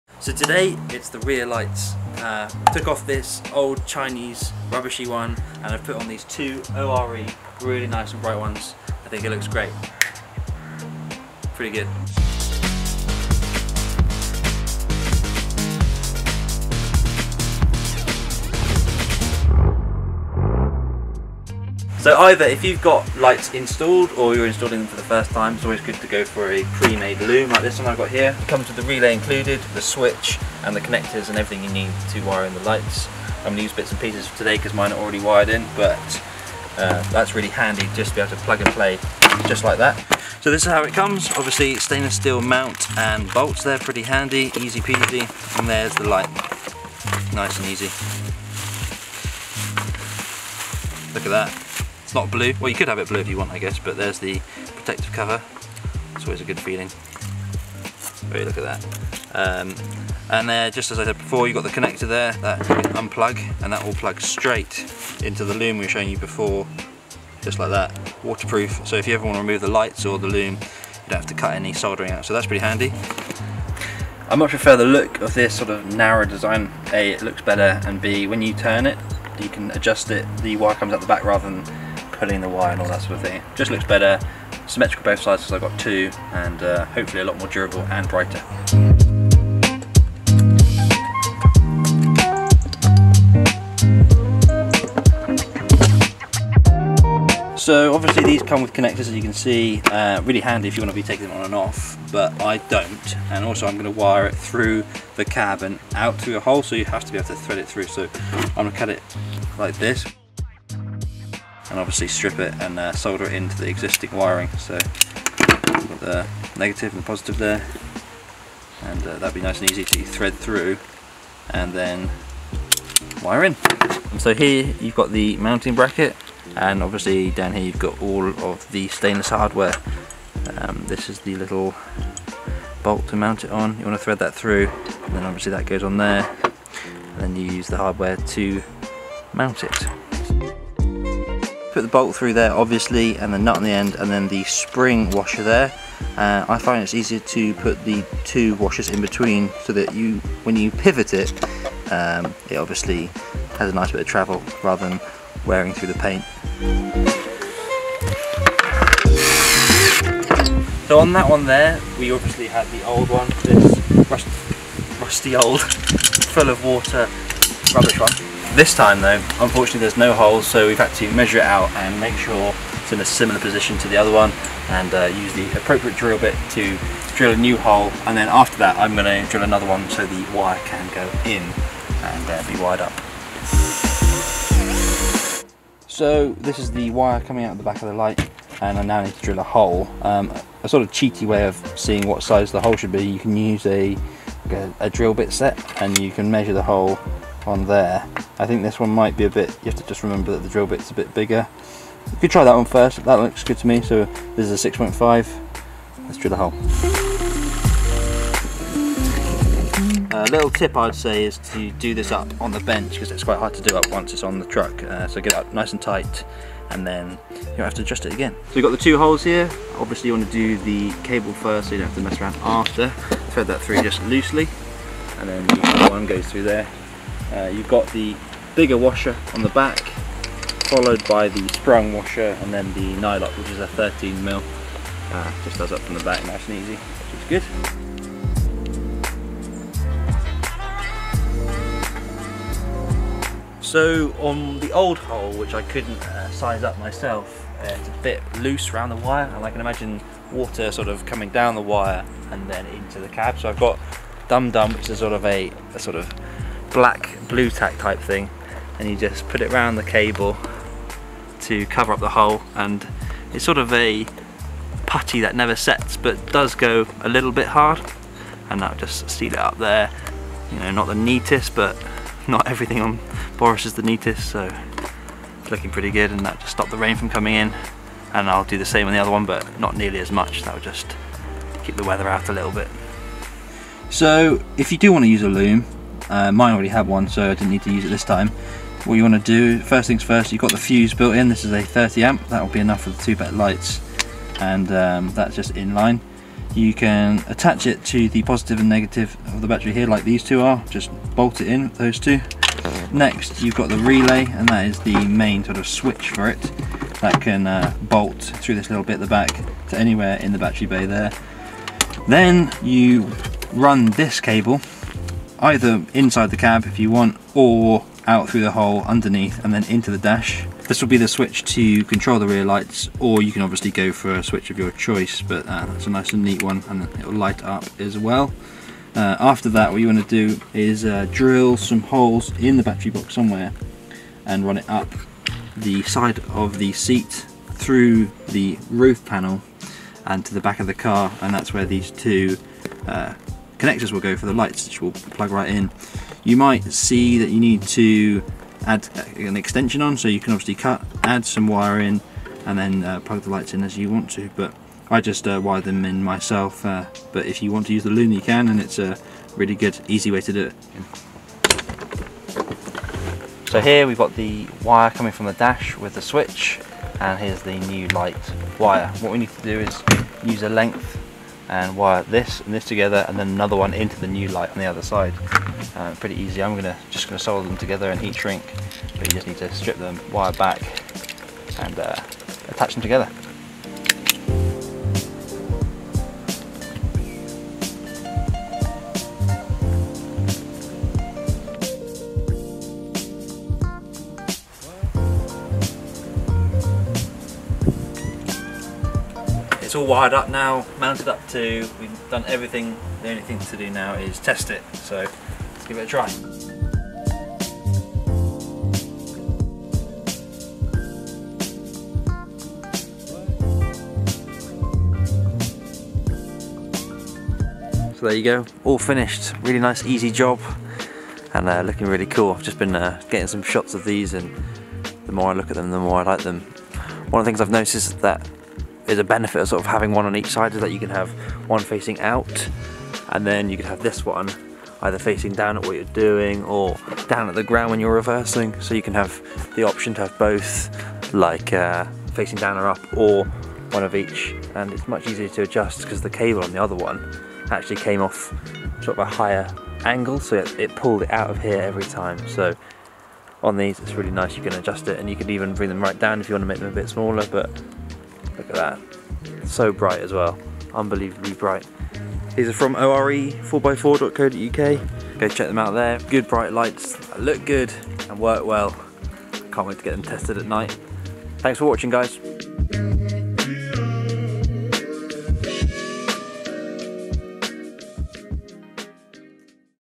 Installing the O.R.E LED Work Lights On my Defender
Do you like being able to see while reversing or working behind your landrover? So do I! With 2 of these lights installed I couldnt believe the difference to the cheaper options. Brighter, more flexible and a better build quality. Very happy to have these on the back of Boris.